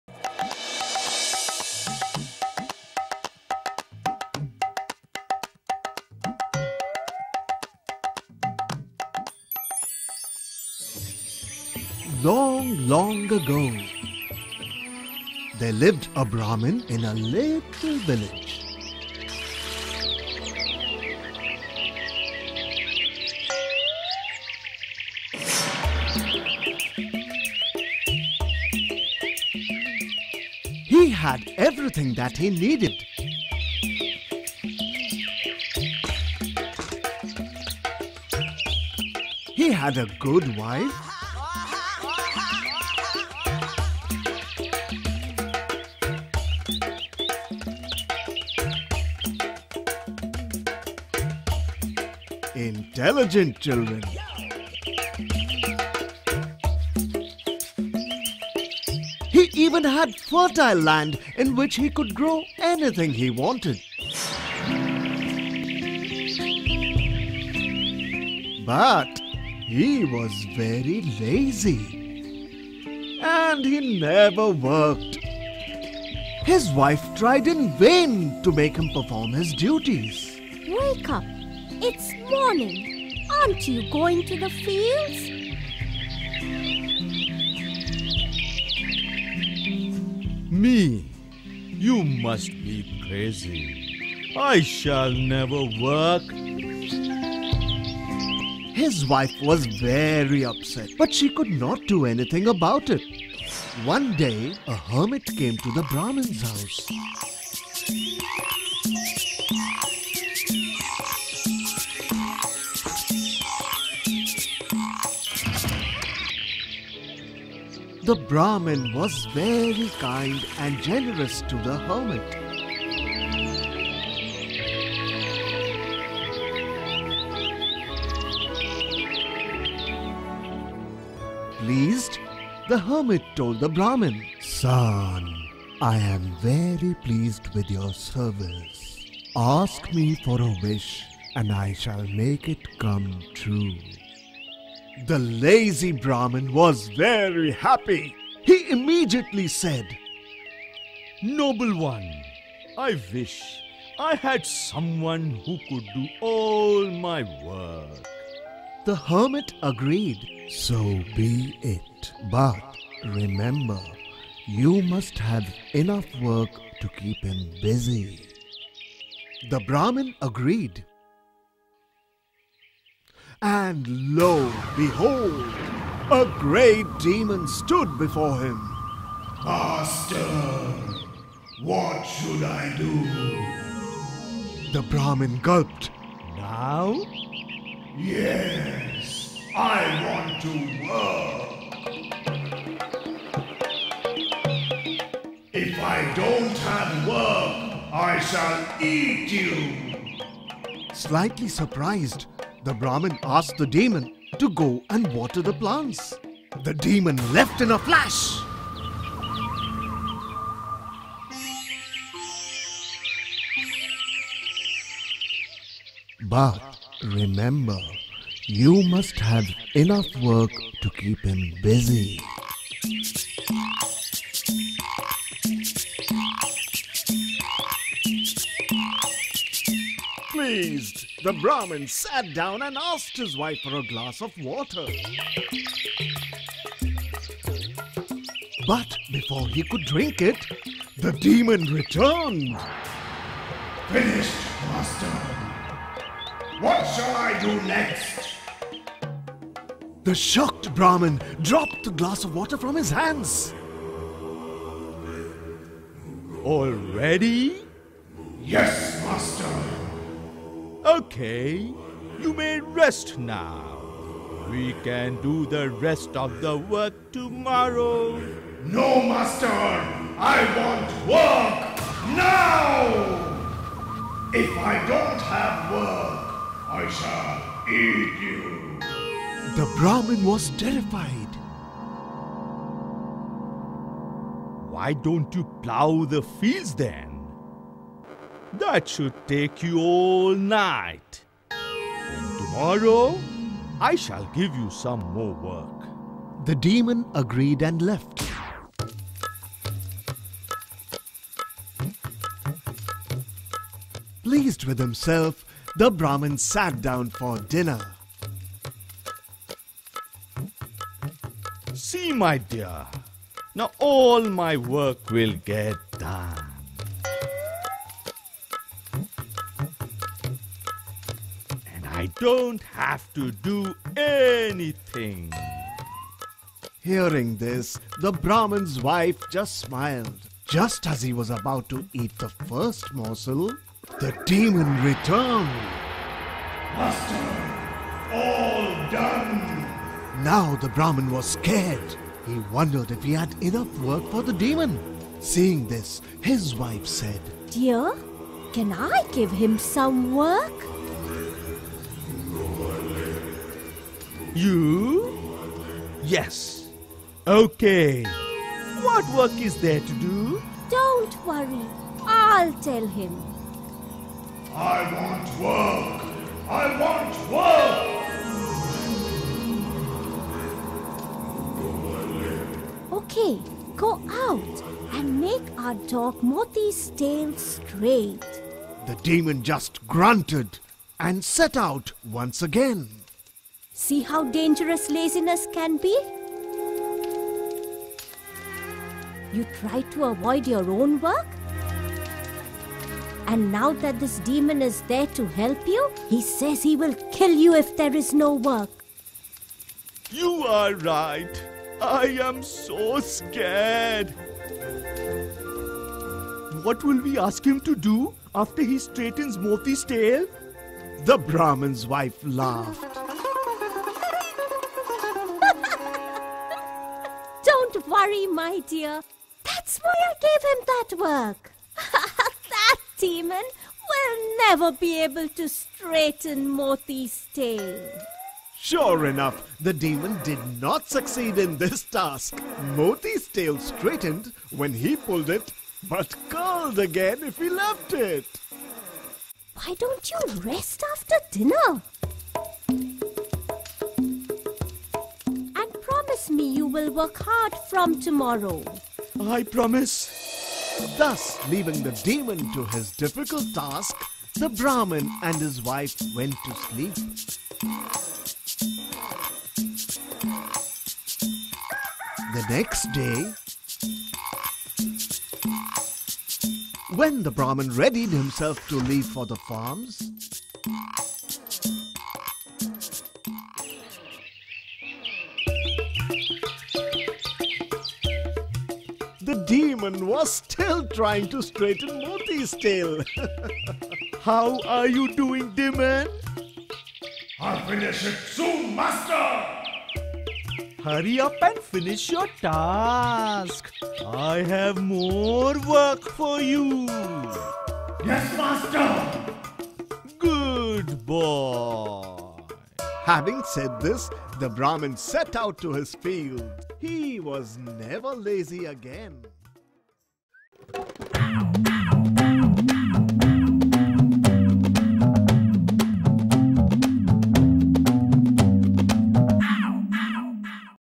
The Lazy Brahmin - Moral Stories for Kids - English
In a village lived a Brahmin with his family. But he never worked due to his laziness. One day a Hermit visited his house and was pleased by the Brahmin, so the Hermit on Brahmin's wish gifted him a great demon saying, "The demon should have enough work to do other wise he will eat you up". Will the Brahmin be able to save his life?"